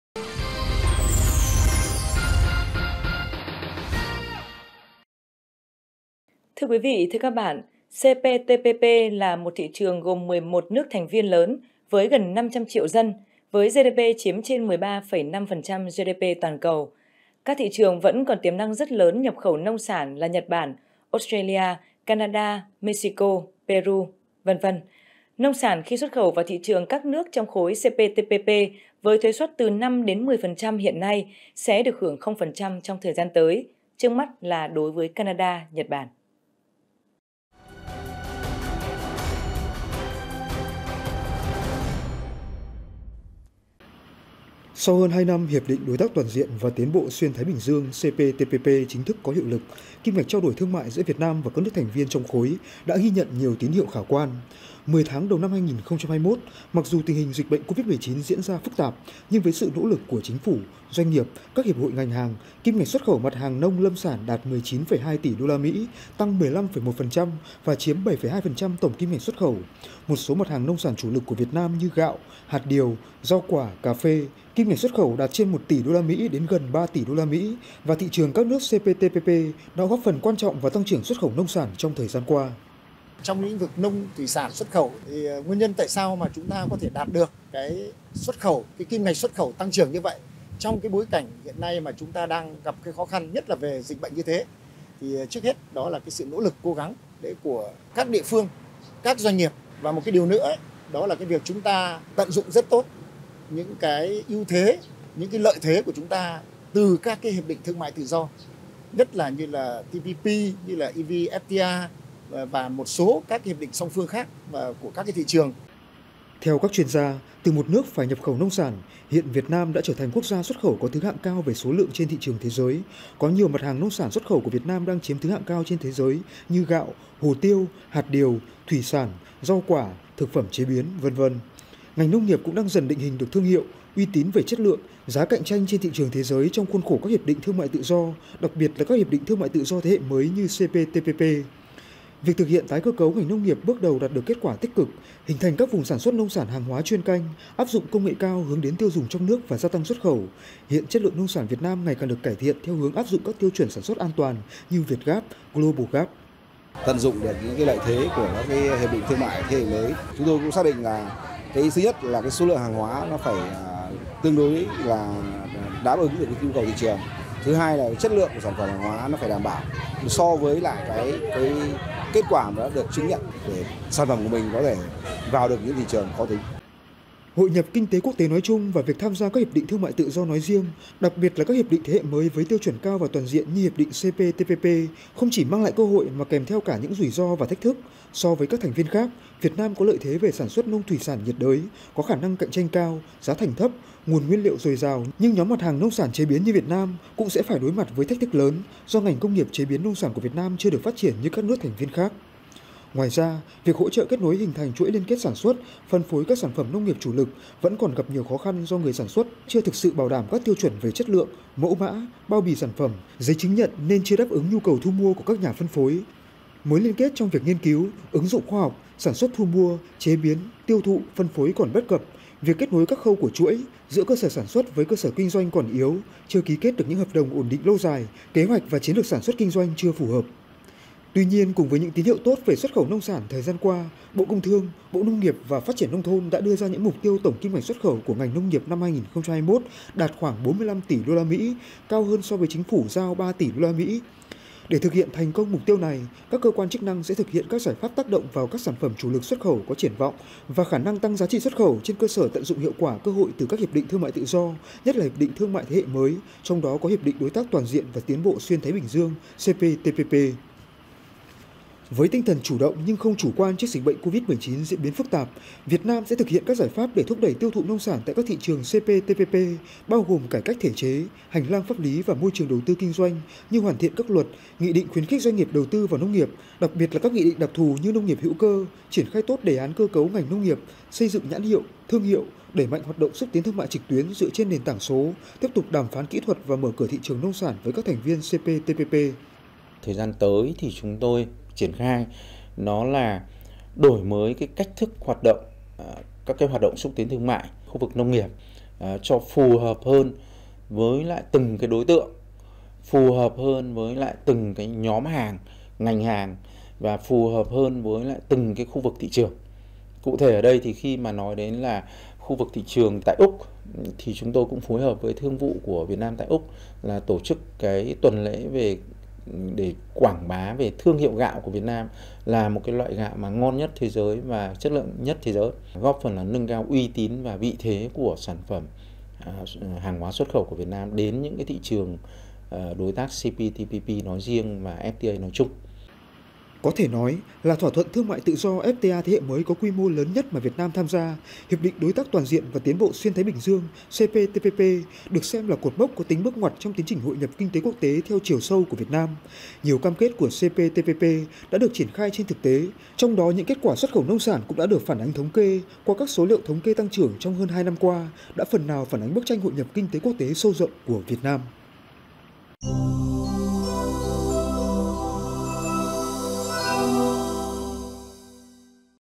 Tận dụng CPTPP đẩy mạnh xuất khẩu nông sản
CPTPP là một thị trường gồm 11 nước thành viên lớn với gần 500 triệu dân với GDP chiếm trên 13,5% GDP toàn cầu. Các thị trường vẫn còn tiềm năng rất lớn nhập khẩu nông sản là Nhật Bản, Australia, Canada, Mexico, Peru... Nông sản khi xuất khẩu vào thị trường các nước trong khối CPTPP với thuế suất từ 5-10% hiện nay sẽ được hưởng 0% trong thời gian tới, trước mắt là đối với Canada, Nhật Bản...